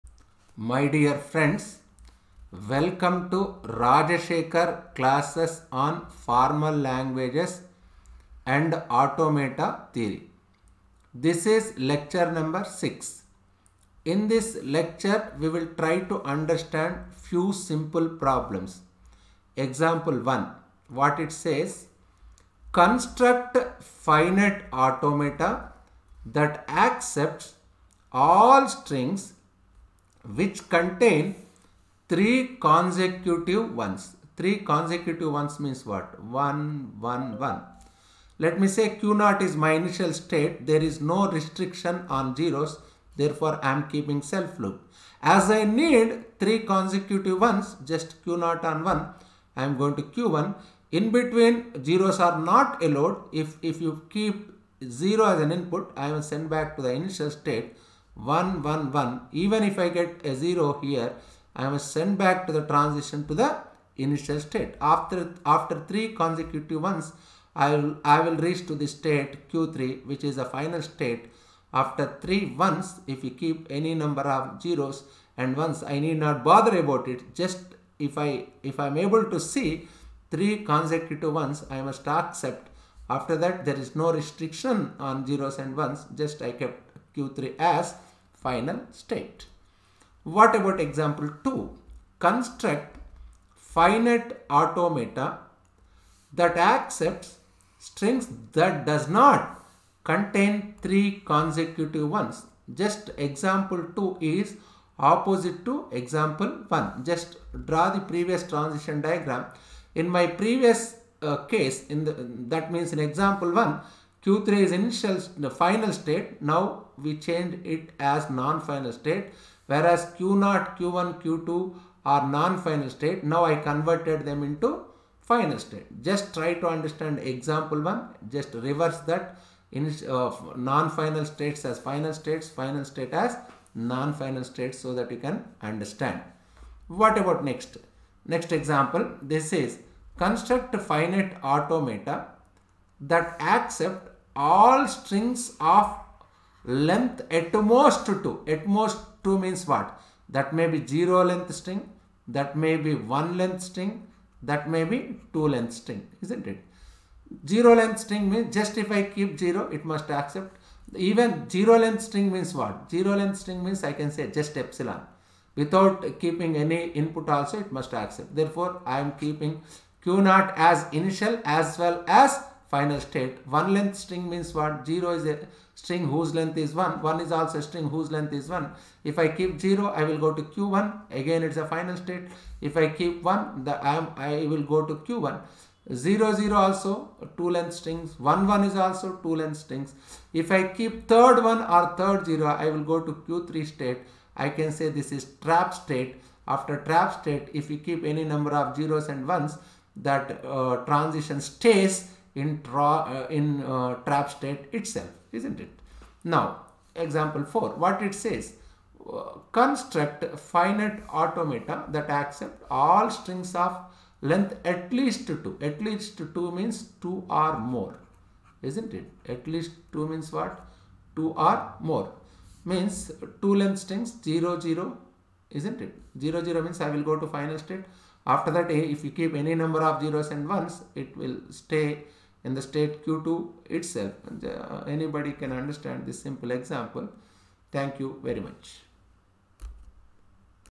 Construction Finite Automata FLAT LECTURE#6
Construct Finite Automata that accepts all strings which contains three consecutive 1’s over ∑ = {0, 1}
Construct Finite Automata that accepts all strings that does not contains three consecutive 1’s over ∑ = {0, 1}
Construct Finite Automata that accepts all strings of length atmost2 over ∑ = {0, 1}